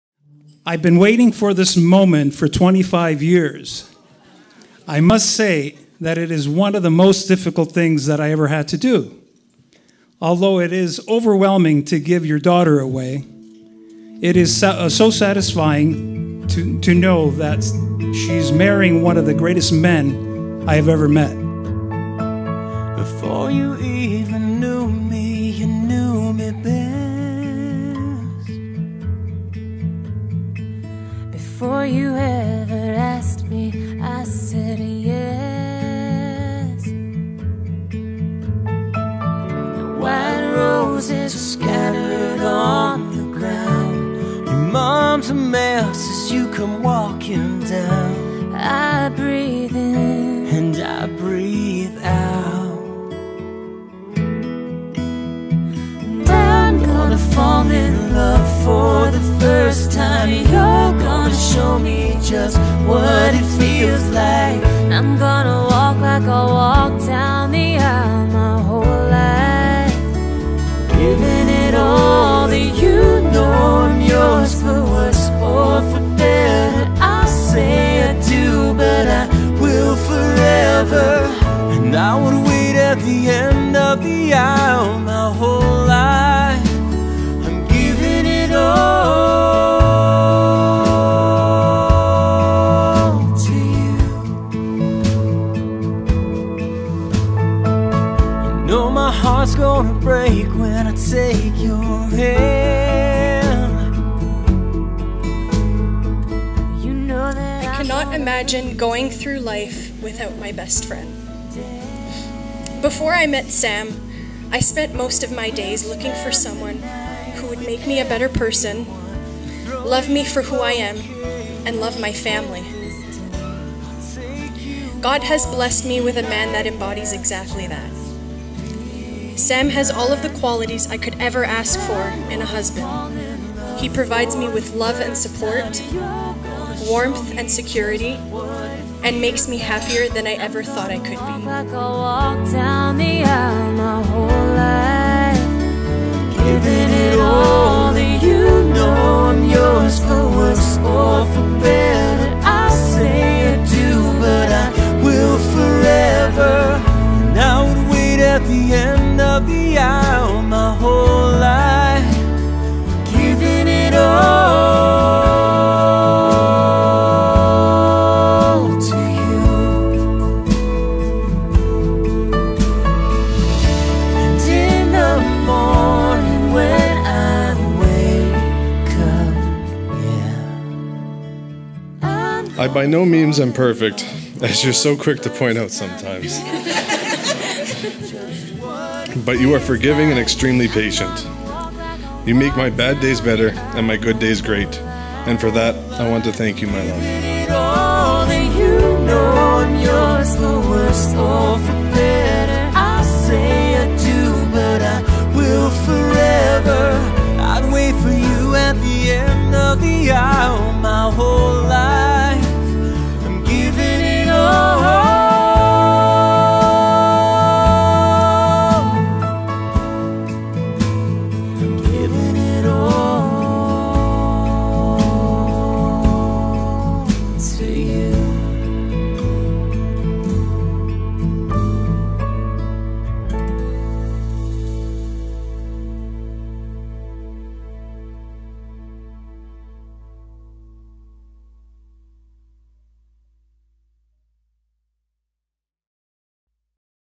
Bradford Barn Wedding Highlight
This wedding took place in Bradford, Ontario at the Bradford Barn The music, the outfits and atmosphere all fit the nice country theme of the wedding.